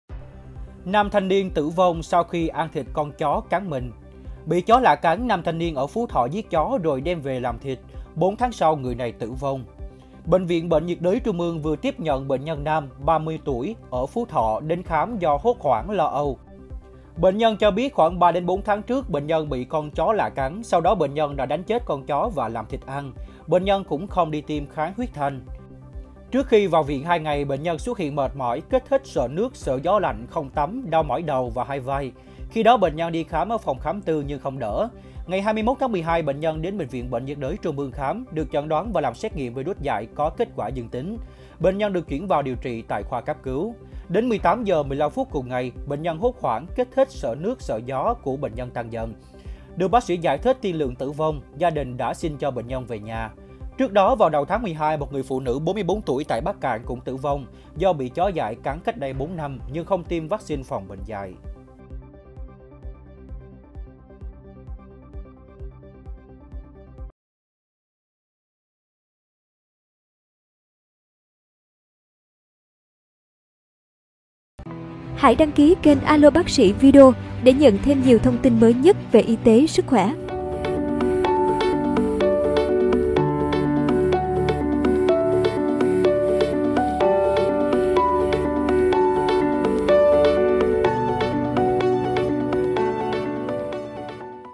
Nam thanh niên t.ử v.o.n.g sau khi ăn thịt con chó cắn mình
Bị chó lạ cắn, nam thanh niên ở Phú Thọ giết chó rồi đem về làm thịt, 4 tháng sau người này tử vong.👉 Tin tức COVID-19 mới nhất

MỌI GÓP Ý XIN GỬI VỀ:
Hợp tác nội dung:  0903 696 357